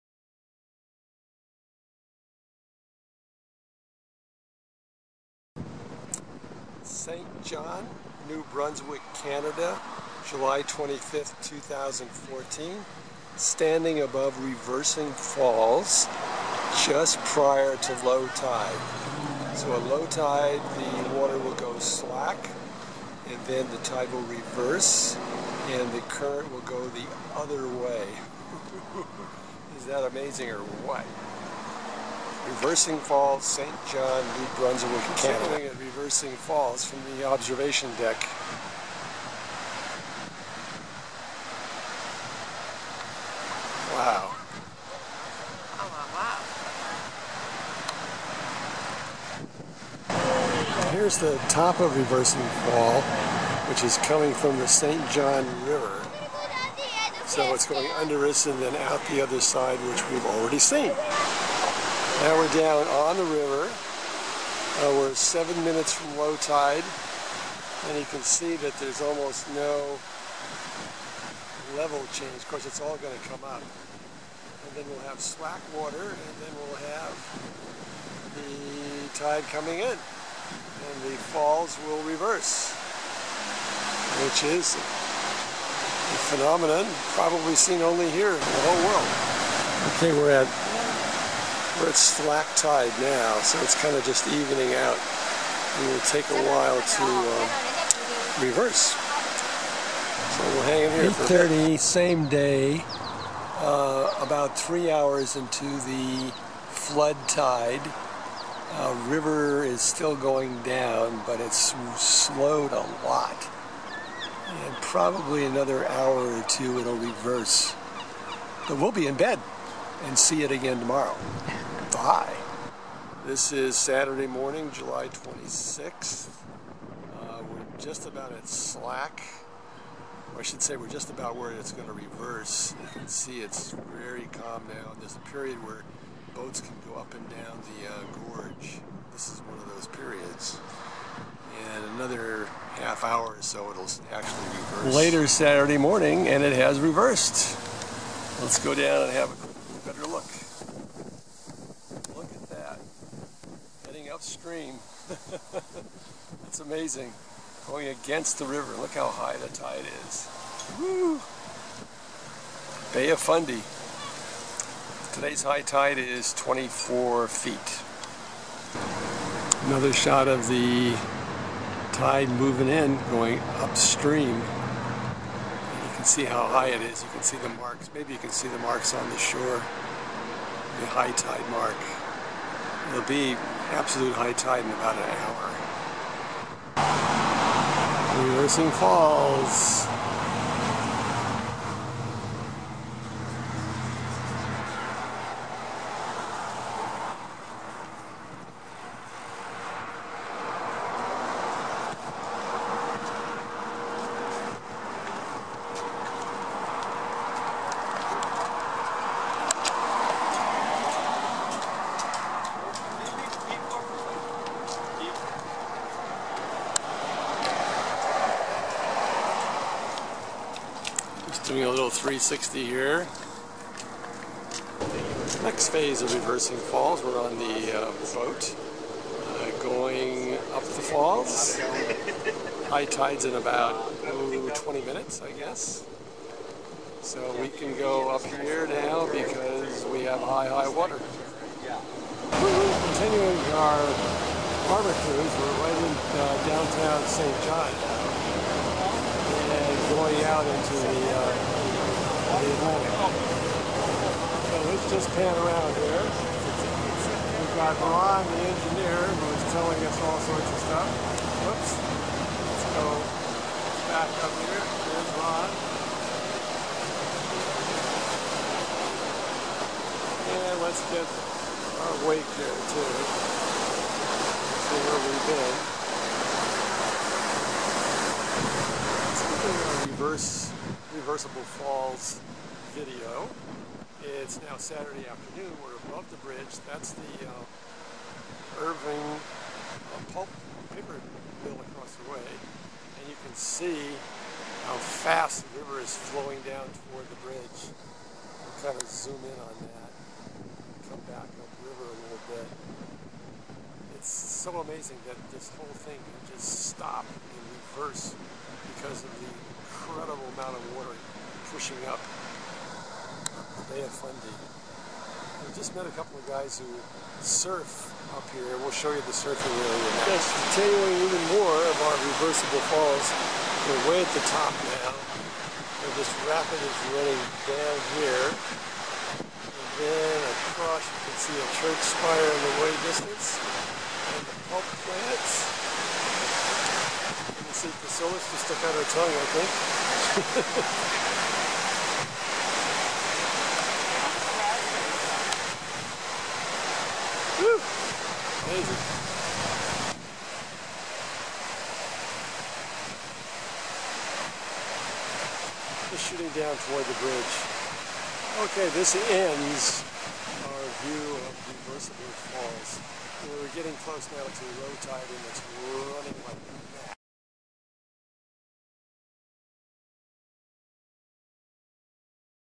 Reversing Falls St John, NB, Bay of Fundy
Reversing Falls is a phenomenon in St. John, New Brunswick, on the Bay of Fundy, where the massive downstream current of the St. John River reverses direction at high tide. The day we shot the video the high tide was 24 feet.